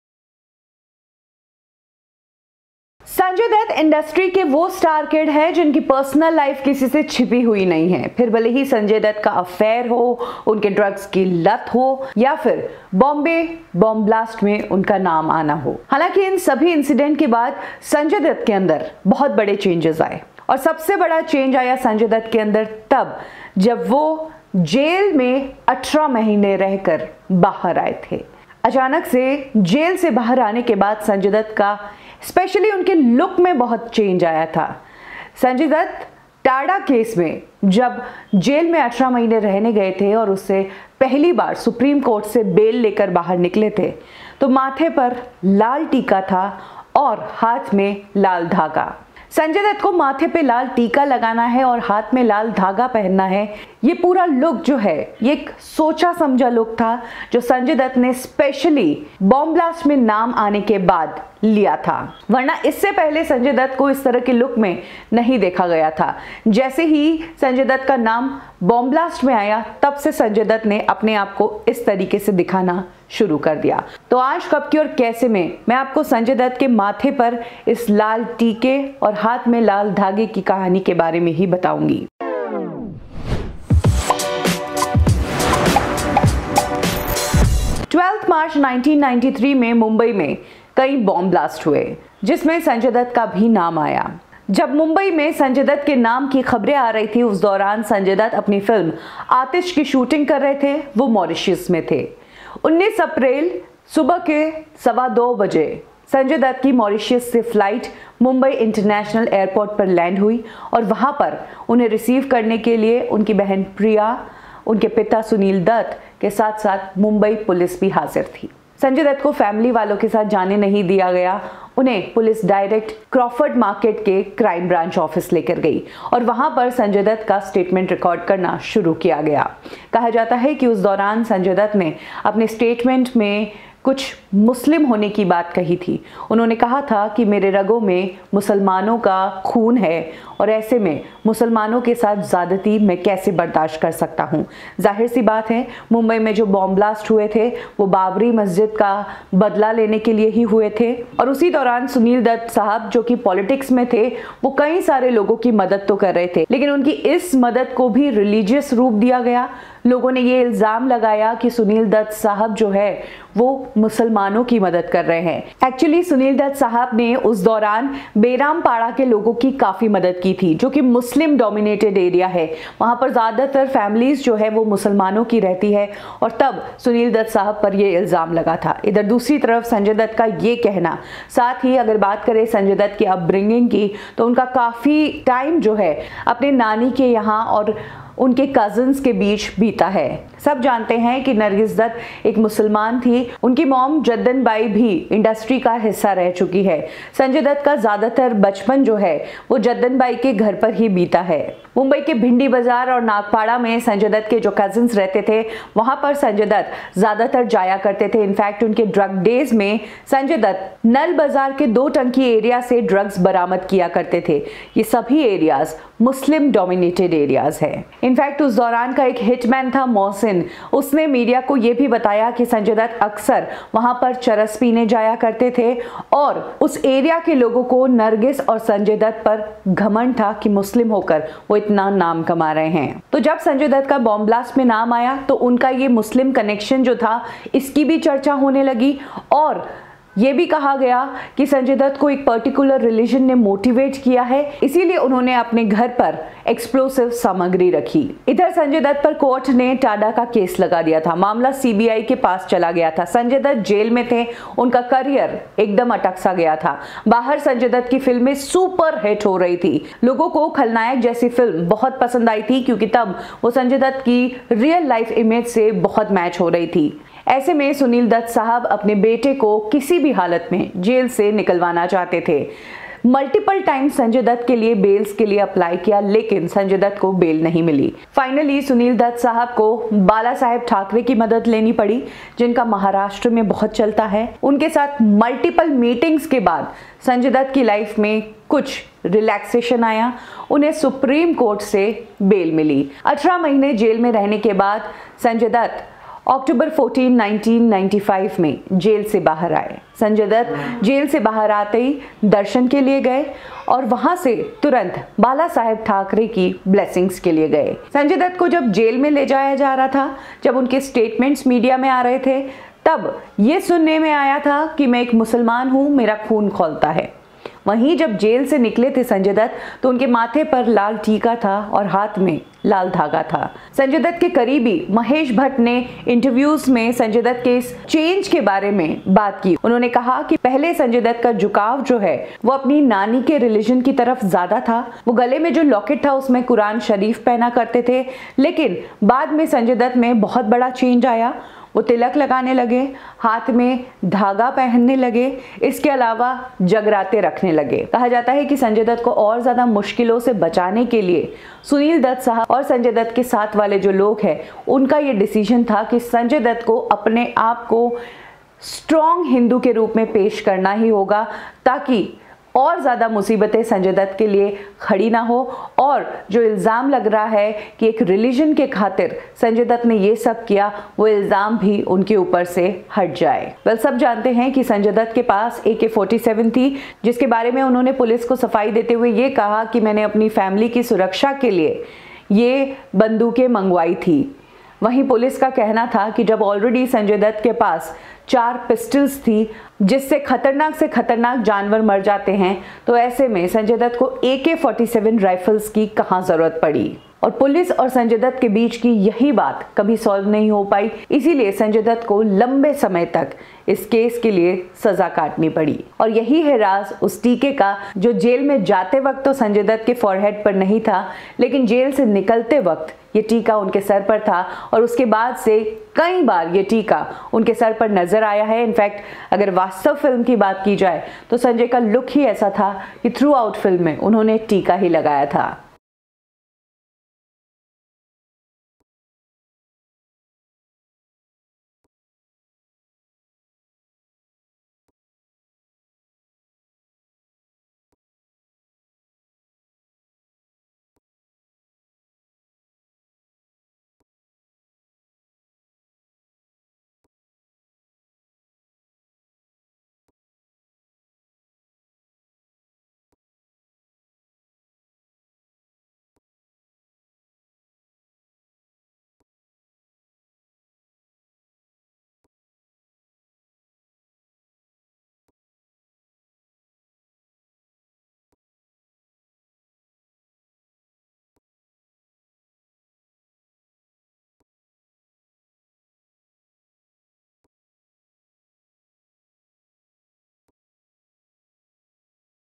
Dark Secret | क्या हुआ था 1992 में ऐसा की Sanjay Dutt लगाने लगा था अपने माथे पे लाल रंग टिका ?
Dark Secret | क्या हुआ था 1992 में ऐसा की SanjayDutt लगाने लगा था अपने माथे पे टिका ? ये है असली कहानी